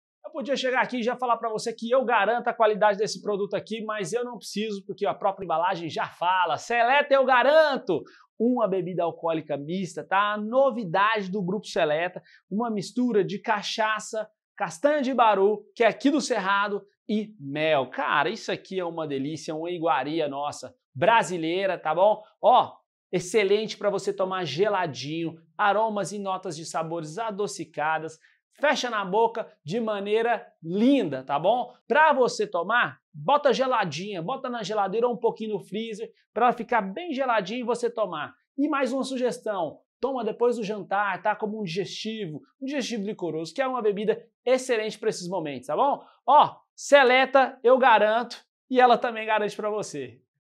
SELETA EU GARANTO
Para quem aprecia o lado doce da vida.

SELETA EU GARANTO é mais uma das criações de Antônio Rodrigues, o Toni, como é carinhosamente chamado pelos amigos. Foi em 2010, em uma de suas muitas andanças pelo Brasil afora, na cidade de Cristalina, que o fundador da Seleta conheceu o baru, um fruto nativo do Cerrado, famoso pelo sabor e pelas suas propriedades energéticas de nutrientes.

Sabendo disso, teve a brilhante ideia de misturar o fruto à sua cachaça e criar um novo  produto com o selo de qualidade SELETA.

GRADUAÇÃO ALCÓOLICA 33%
ARMAZENADA EM UMBURANA
TEMPO DE ARMAZENAMENTO 2,5 ANOS

Para quem aprecia a garantia de uma vida com mais sabor.

Foram anos preparando e aprimorando a receita, que, além do baru, leva mel na medida certa. O resultado foi a SELETA EU GARANTO! Uma mistura da preciosa Cachaça SELETA Umburana com baru e mel. Com teor alcoólico inferior ao das cachaças tradicionais e um sabor adocicado e agradável aos mais diversos paladares, tem um quê de ‘‘quero mais’’. SELETA EU GARANTO deve ser apreciada preferencialmente gelada, para que seu sabor seja ainda mais ressaltado.

Garantia de boas sensações em todos os sentidos.

APARÊNCIA
Cor dourado intenso e brilhante.

AROMAS
Possui aroma suave, levemente adocicado, com notas de castanhas, frutas e com um toque leve de baunilha e favos de mel.

SABOR
Seu sabor é encorpado e intenso com notas de mel e frutas.

COMBINA COM O QUÊ?
Combina com bons pratos e bons momentos. Seu sabor mais adocicado cai muito bem como um aperitivo pós refeições ou mesmo acompanhando sobremesas.

MISTURA COM O QUÊ?
Pra quem acha que bebidas mistas só devem ser consumidas puras, vai se surpreender com a versatilidade da SELETA EU GARANTO. Sua combinação pra preparação de drinks e coquetéis é a satisfação garantida para quem  aprecia uma bebida saborosa.

🔞 A venda de bebidas alcoólicas é proibida para menores de 18 anos. Beba com moderação.